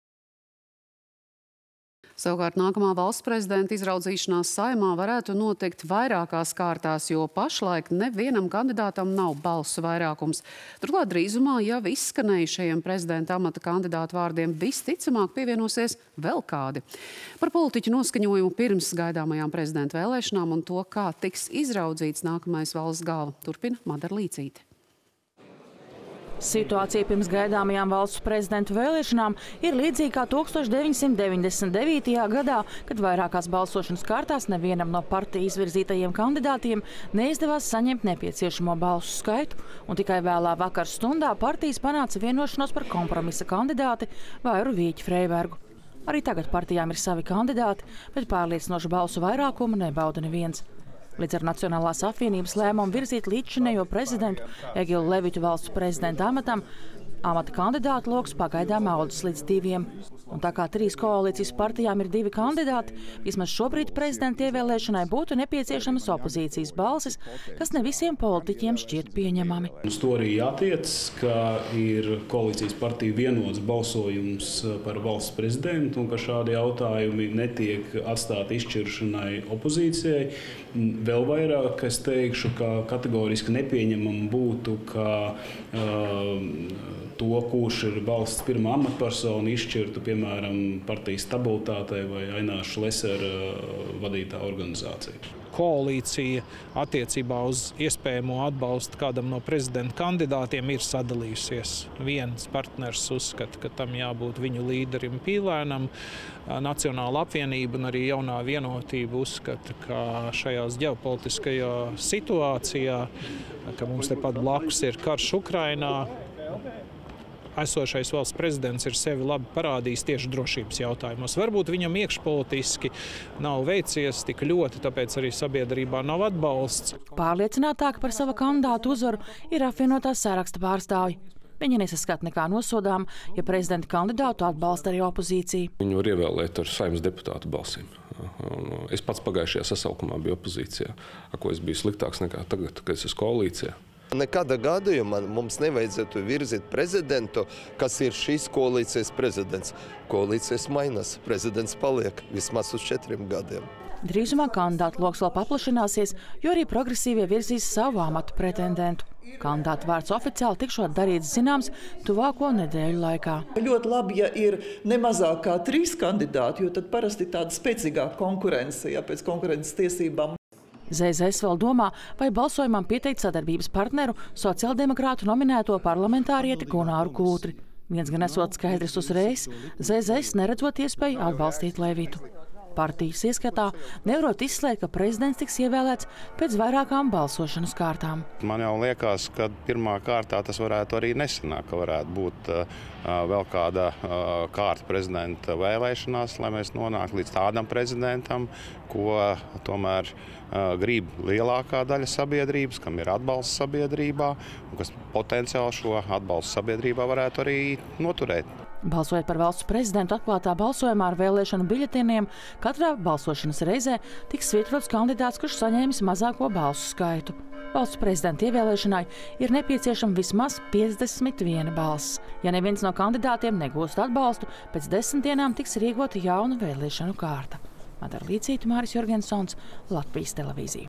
Pieļauj prezidenta ievēlēšanu vairākās kārtās
Nākamā Valsts prezidenta izraudzīšanās Saeimā varētu notikt vairākās kārtās, jo pašlaik nevienam kandidātam nav balsu vairākums. Turklāt drīzumā jau izskanējušajiem prezidenta amata kandidātu vārdiem, visticamāk, pievienosies vēl kādi. Par politiķu noskaņojumu pirms gaidāmajām prezidenta vēlēšanām un to, kā tiks izraudzīts nākamais valsts galva, turpina Madara Līcīte.